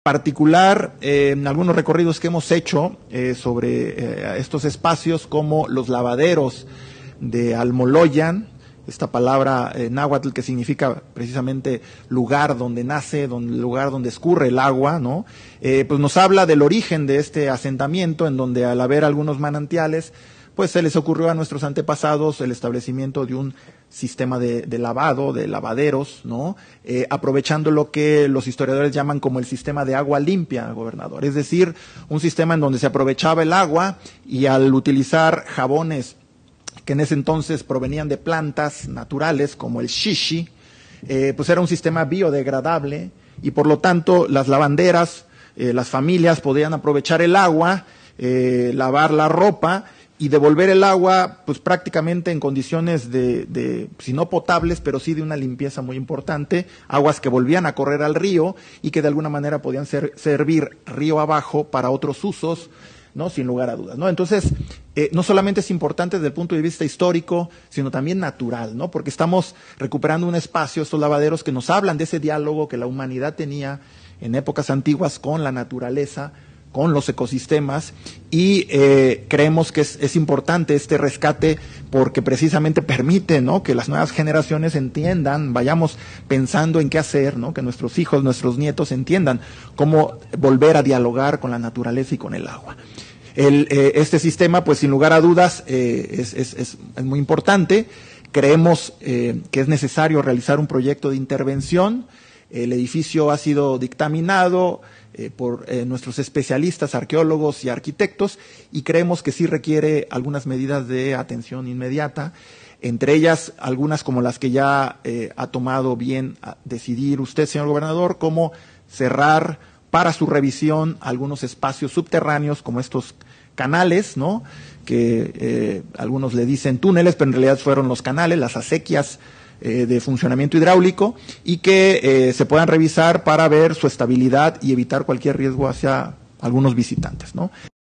Túneles de Puebla serán cerrados para revisar su estabilidad estructural y evitar posibles daños
El delegado del INAH, Manuel Villarruel Vázquez, destacó el proyecto de recuperación de espacios públicos y monumentos históricos en Puebla, incluidos los Lavaderos de Almoloya.